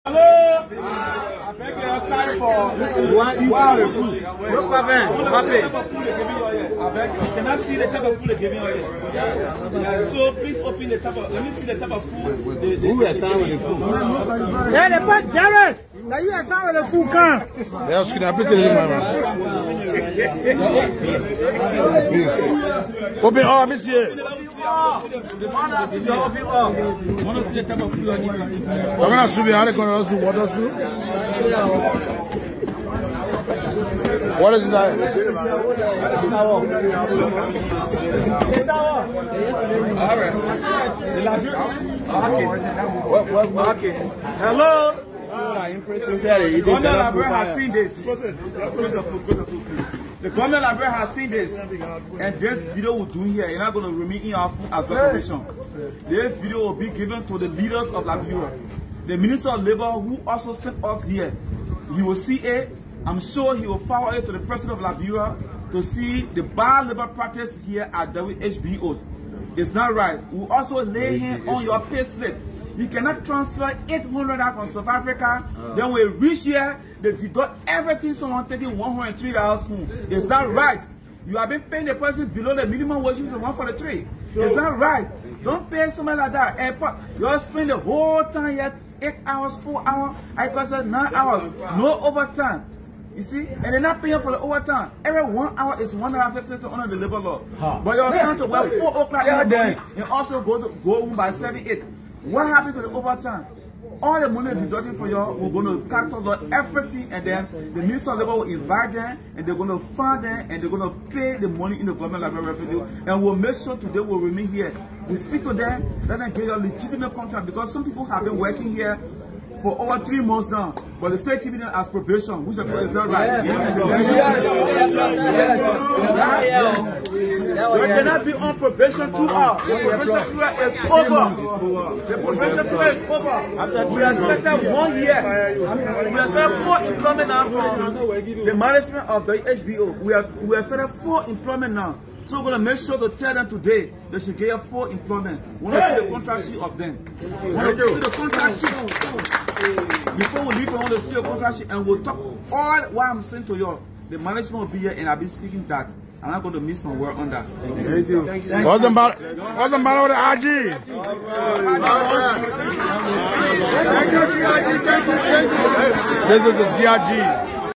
Investigation by Labor Ministry Reveals How Liberians are Treated in Their Own Land...
Investigation by Labor Ministry Reveals How Liberians are Treated in Their Own Land... Deputy Inspector General Darius Weamie.